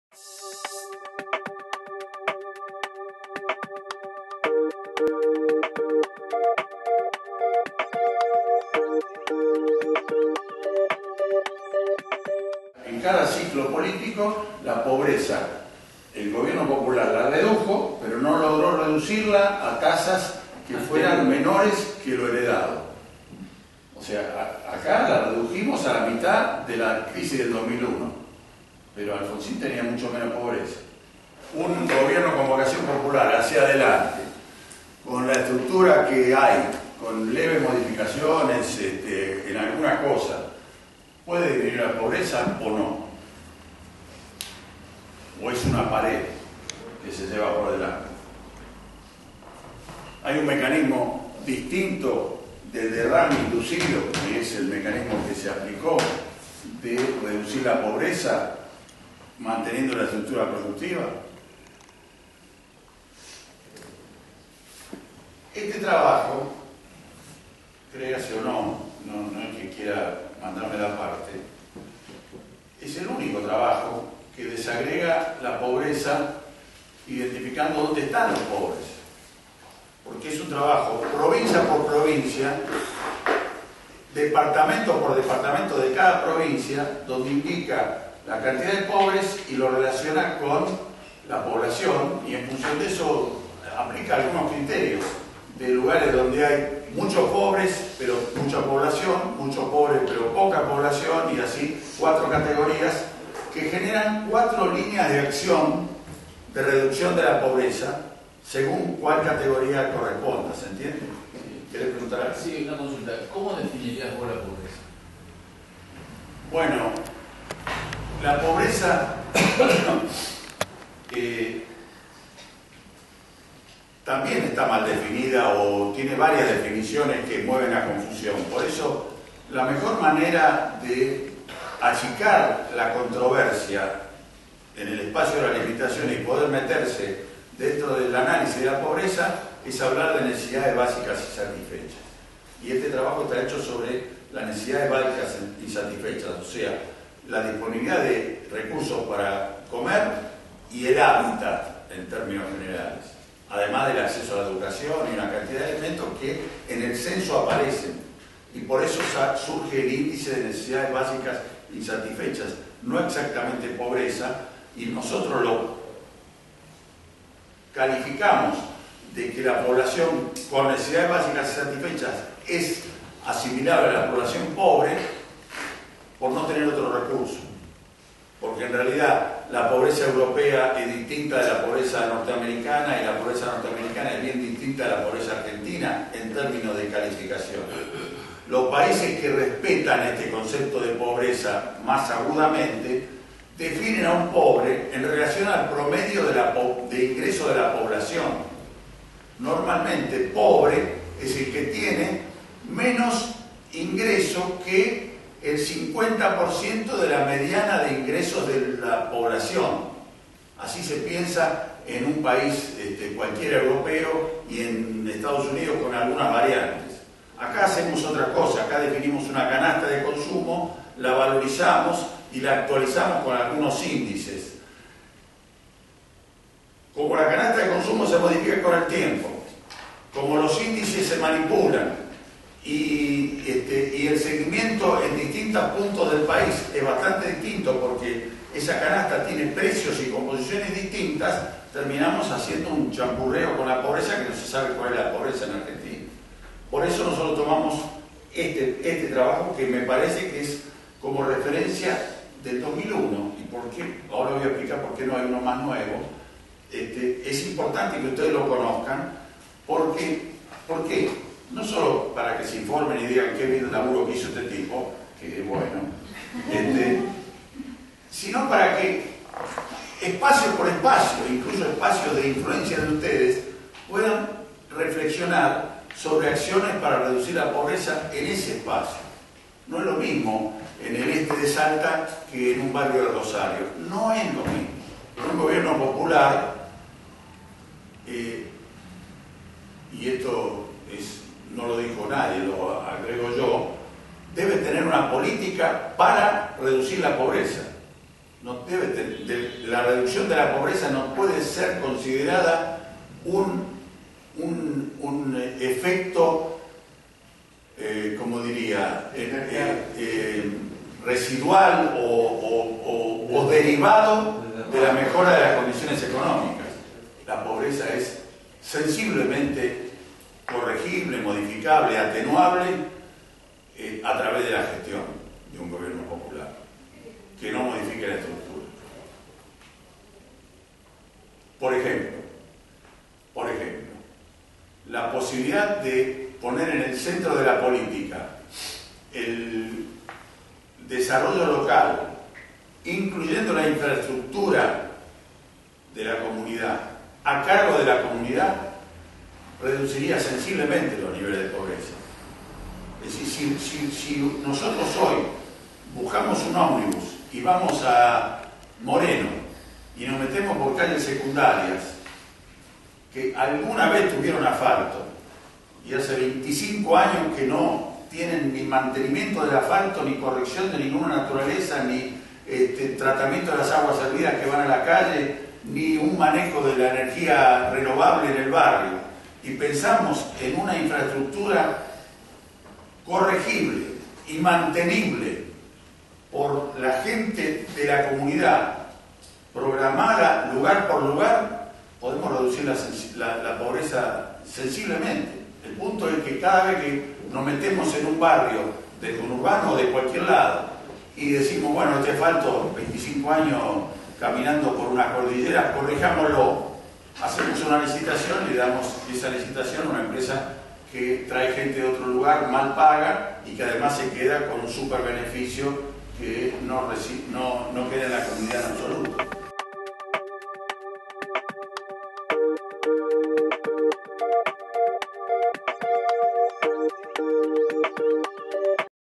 TALLER OCUPÉMONOS. MÓDULO 1. La Pobreza (2/3)
¿Puede ser dismunuida, o erradicada, la pobreza en un Estado de Bienestar, aun con sus límites estructurales? Una pregunta que plantea nuevos interrogantes acerca de las causas que motivan su incremento independientemente del signo político que ocupe el poder en la Argentina contemporánea.